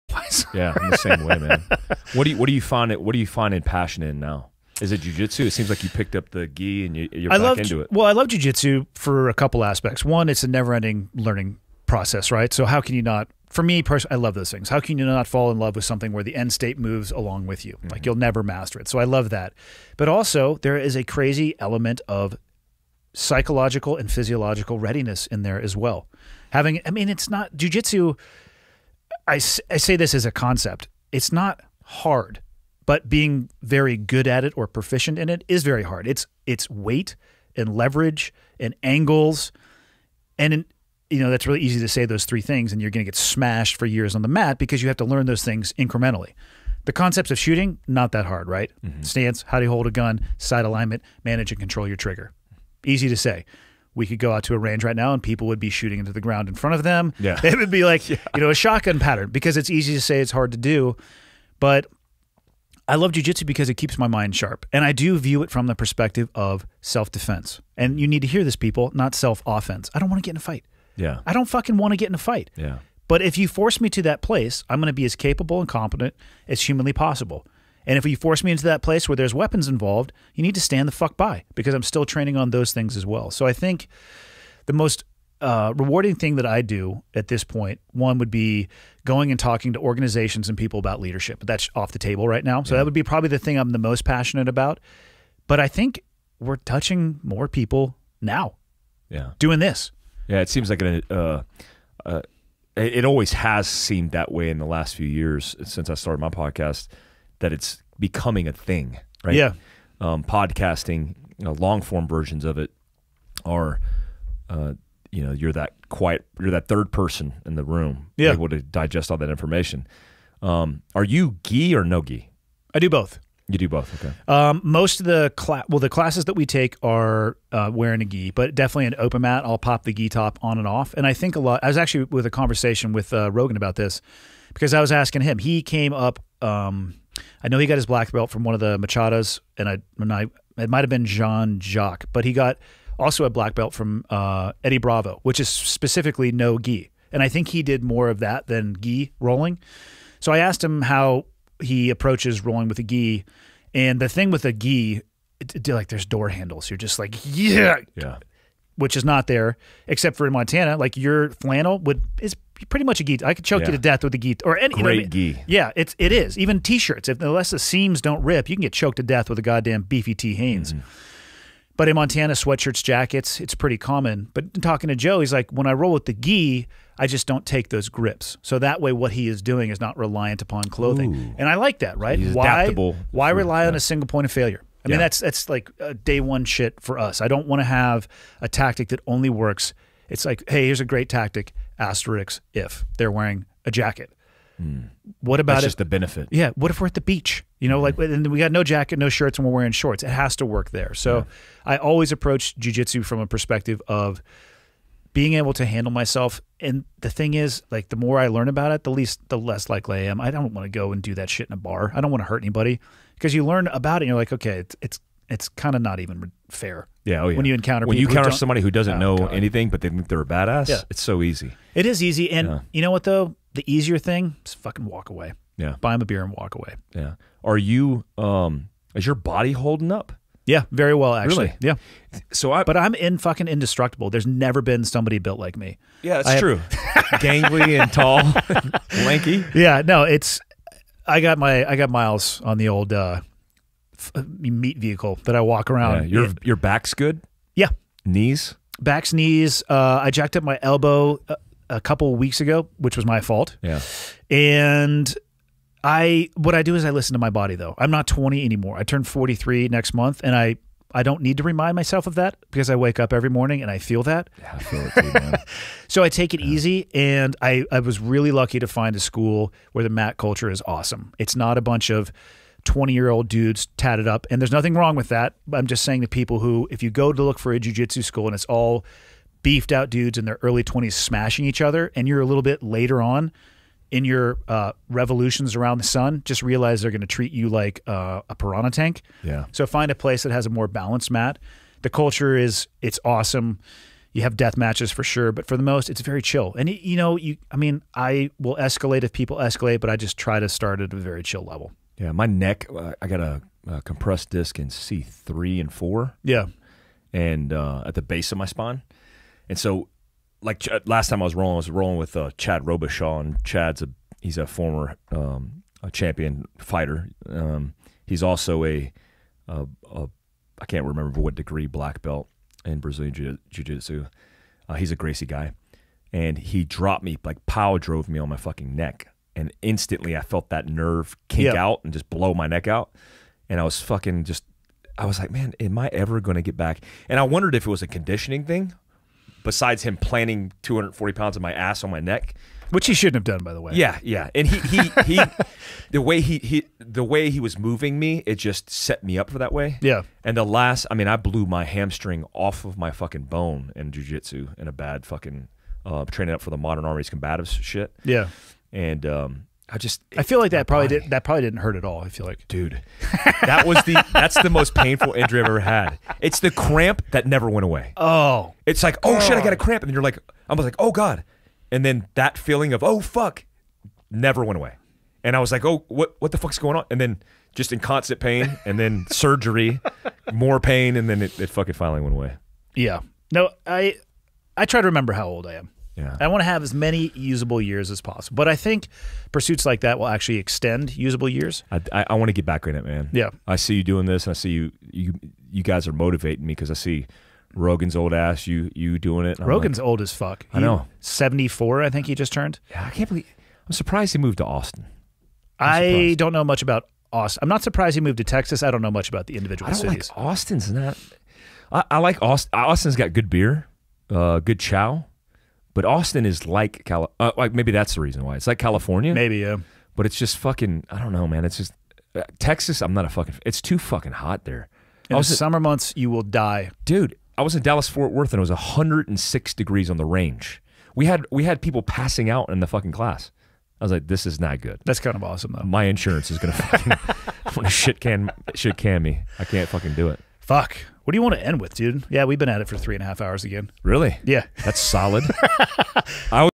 Andy Stumpf and Mike Glover on training BJJ
Great conversation on BJJ with Andy Stumpf and Mike Glover